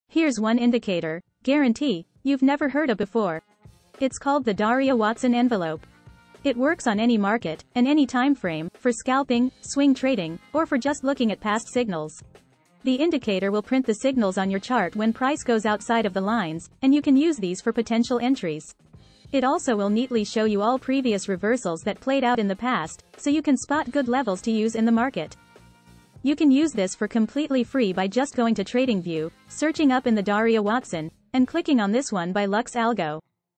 Nadaraya Watson Envelope Trading Indicator (Read pinned comment)
🚀 The most exclusive channel on YouTube for NFT, Crypto and Blockchain related content 😍

Here's one indicator I guarantee, you've never heard of before. It's called the Daria Watson envelope. It works on any market, and any timeframe, for scalping, swing trading, or for just looking at past signals. The indicator will print the signals on your chart when price goes outside of the lines, and you can use these for potential entries. It also will neatly show you all, previous reversals that played out in the past, so you can spot good levels to use in the market. You can use this for completely free by just going to trading view, searching up in the Daria Watson, and clicking on this one by Lux algo.